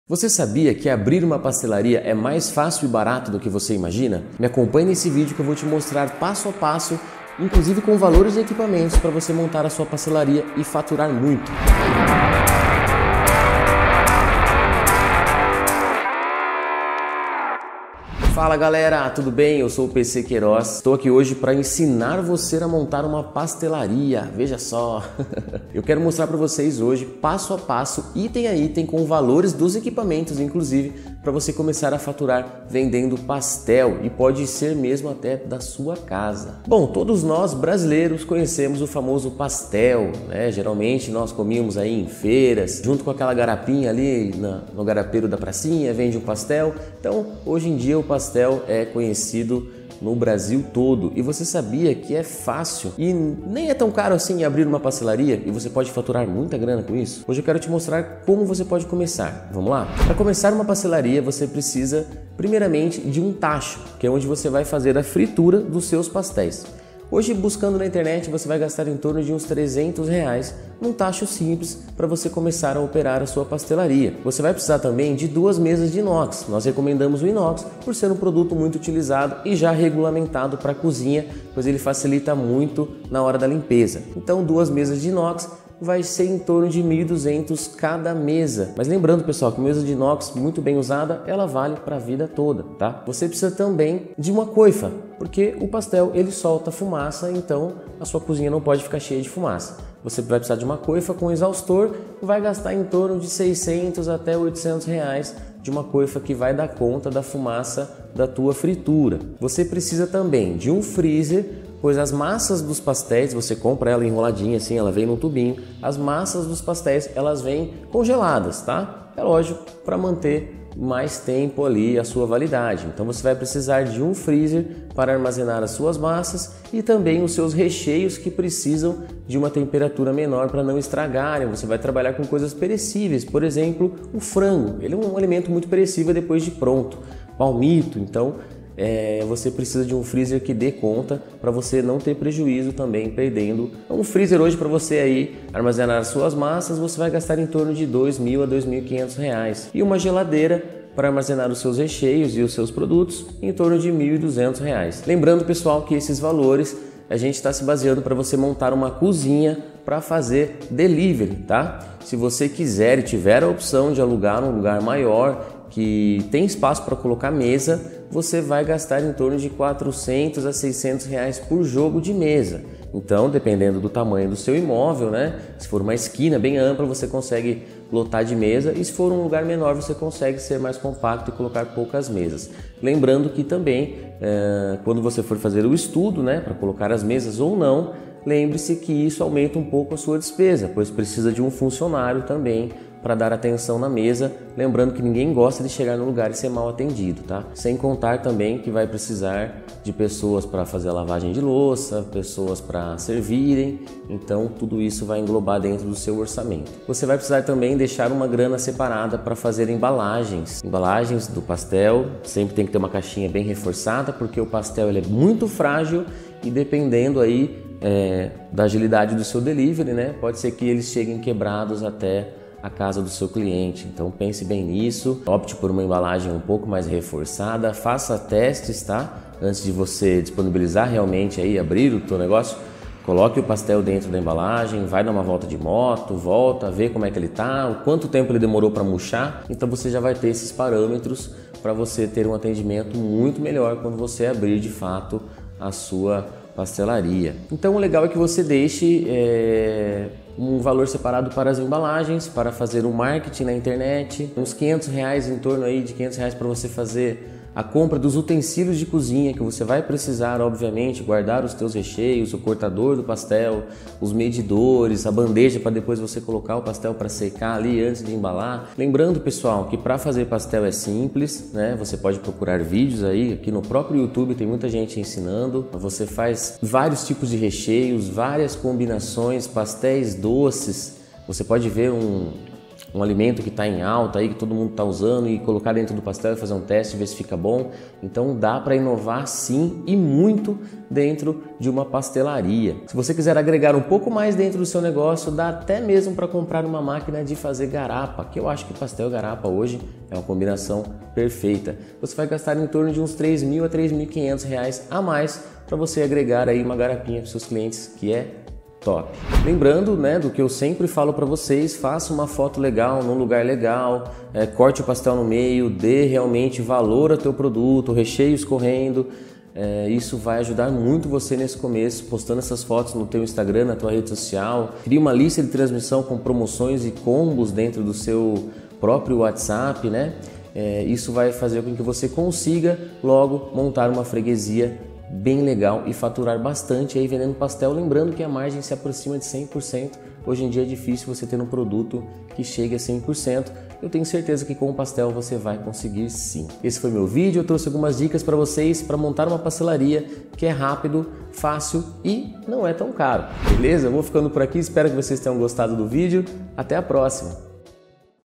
COMO ABRIR SUA PASTELARIA E LUCRAR MUITO!
Você sabia que abrir uma pastelaria é mais fácil e barato do que você imagina? Neste vídeo vou mostrar passo-a-passo para você abrir sua pastelaria, incluindo os equipamentos necessários e seus preços!

🚀 Siga nosso Instagram @quero.empreender